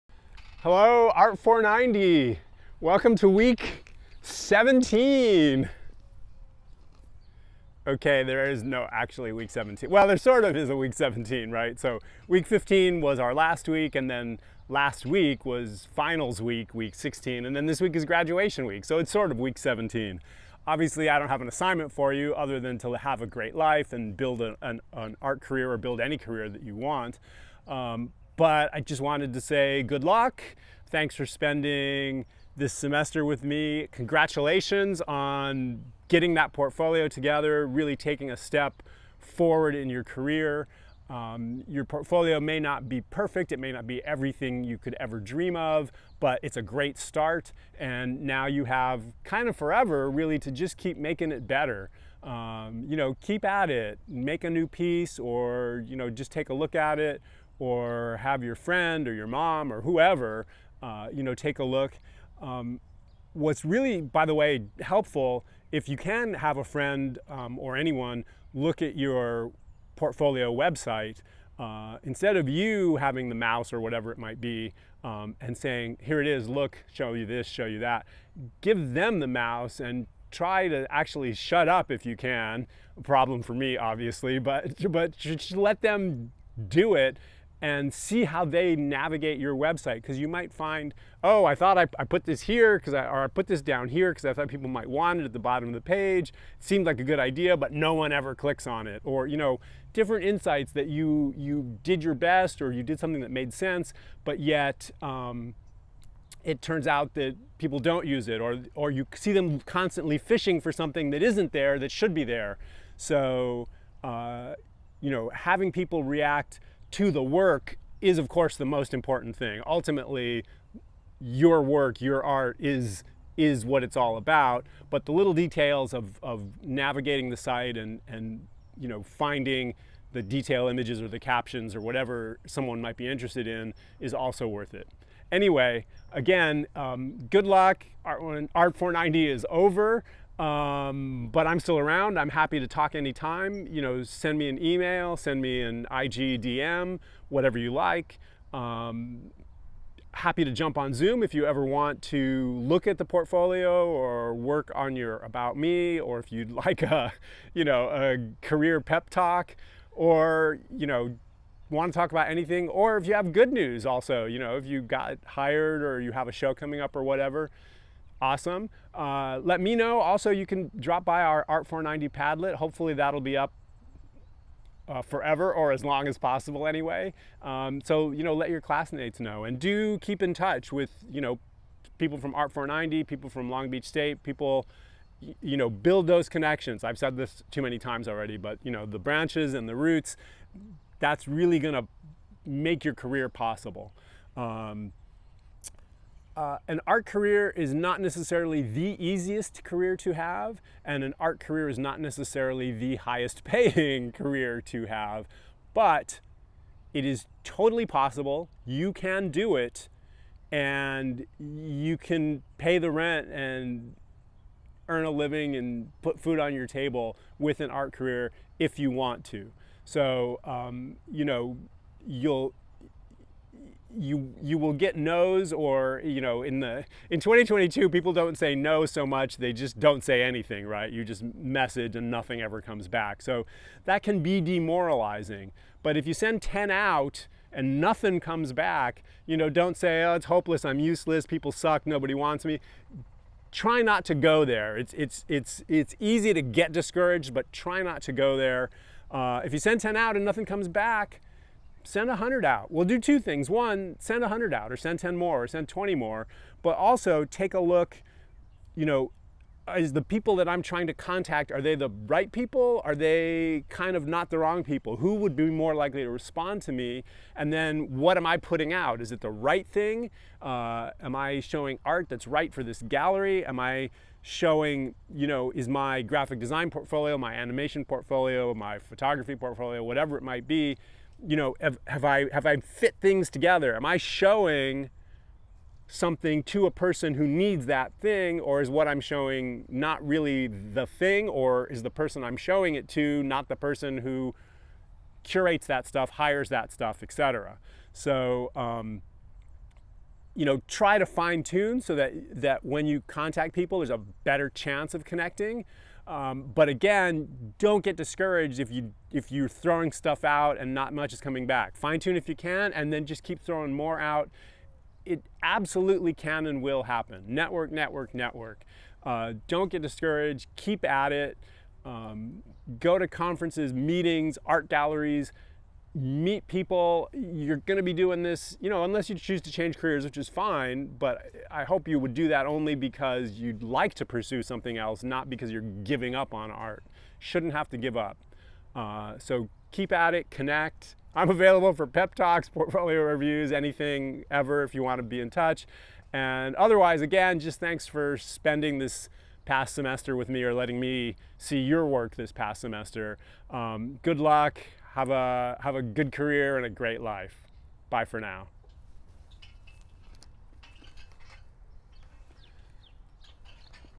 LBSU Art 490 Sp'22 Wk17 - Good Luck!
Thanks for a wonderful semester! Congratulations on your shiny, new portfolio! And best wishes as you head out into the post-SOA world! Shout if I can help with anything.

Art 490 - Capstone Workshop in Artist's Portfolios, Websites & Careers
School of Art
College of the Arts
Long Beach State University